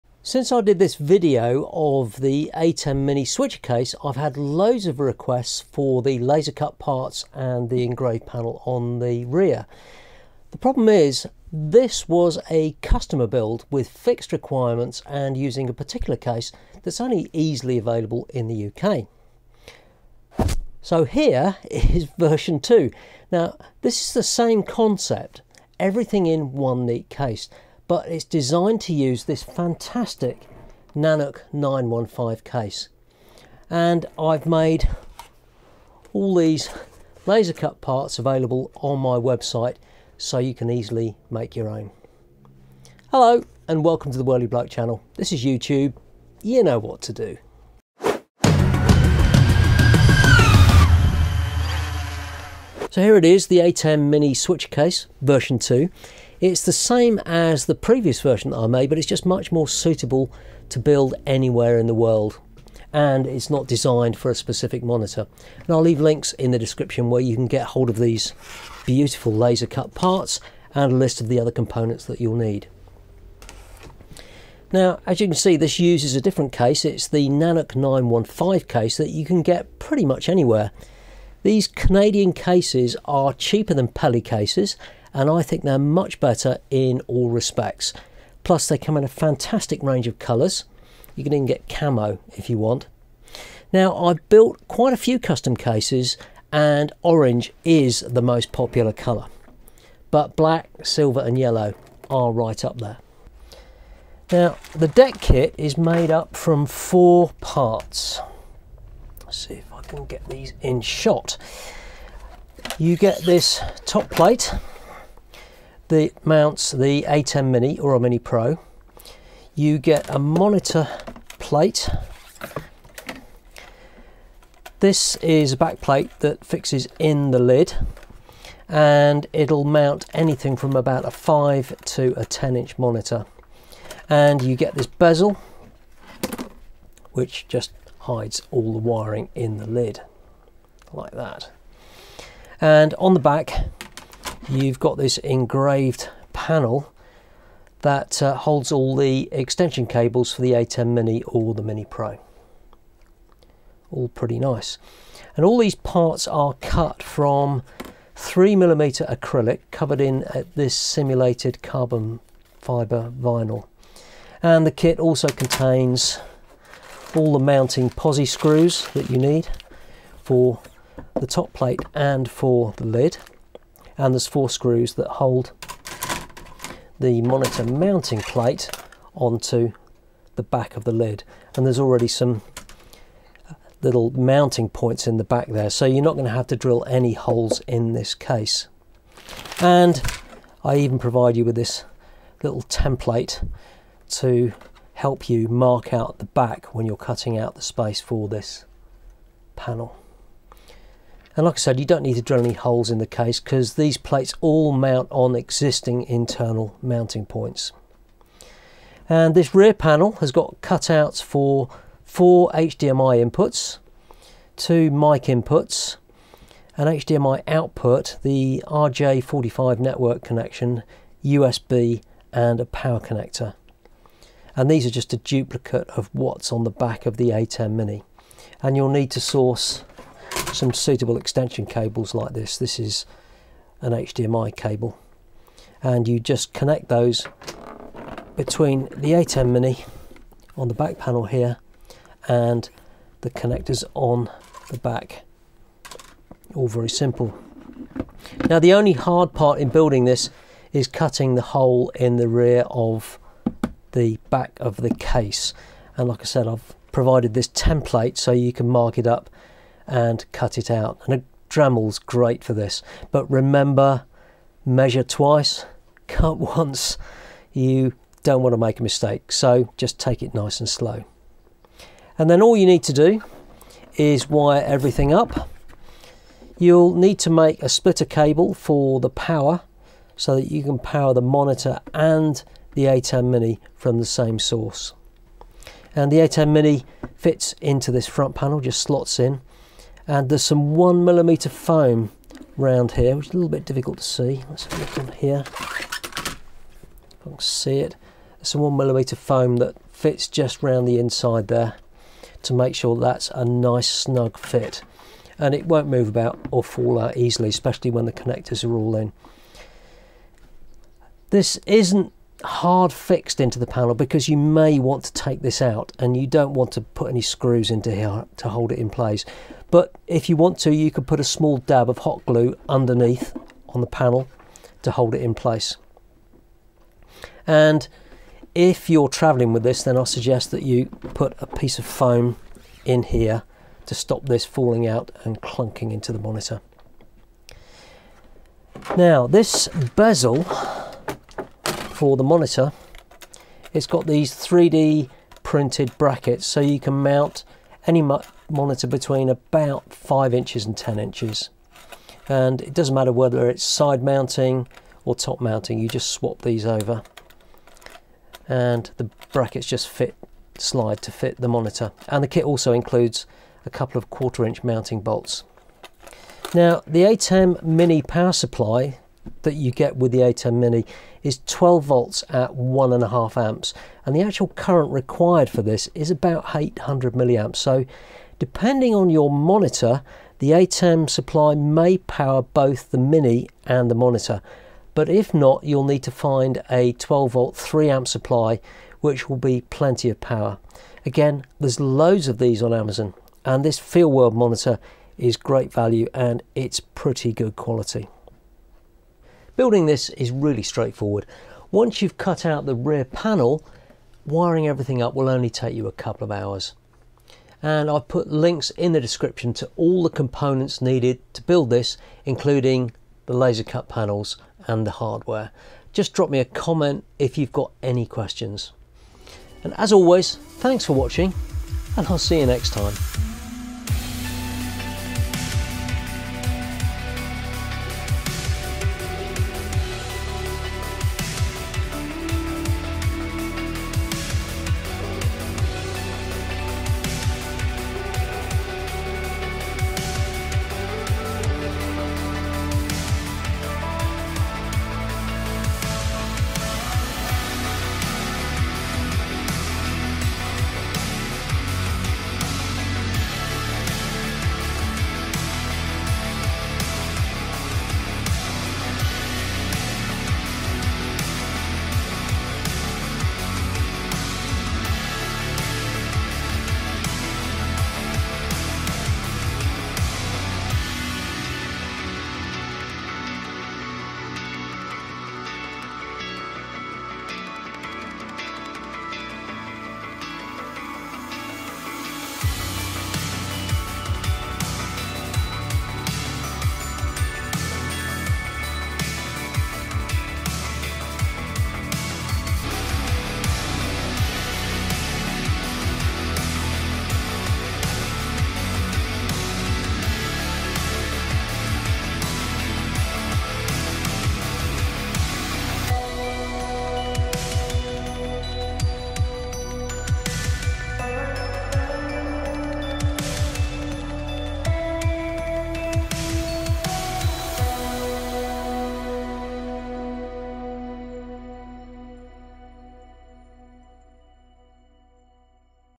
ATEM Mini Switcher Portable Case Version 2 / Switch and Monitor in one portable Nanuk 915 case
Since I did a video of the Portable ATEM Mini switcher case, I've had loads of requests for the laser cut parts and engraved rear panel. The problem is, that was a customer build with fixed requirements and using a particular case that is only easily available in the UK.

So here's version 2. This is the same concept, everything in one neat case, but designed to use the fantastic Nanuk 915 case. And I've made all the laser cut parts available on my website so you can easily make your own.

► Nanuk 915 Case case
► BlackMagic ATEM Mini
► FeelWorld T7 4K Monitor
► HDMI panel cable extensions
► Braided sleeving
► Slim line HDMI cable
► 3.5mm stereo jack socket
► 3.5mm stereo jack plugs and cables
► Panel mount USB-C extension
► HDMI splitter 4K, 1080p

00:00 Welcome the the whirlybloke channel
01:15 The Nanuk 915 case
01:45 The case deck kit laser cut panels
03:30 The rear engraved panel
05:40 Monitor mounting plate
06:00 Power supply requirements
06:39 Thanks for watching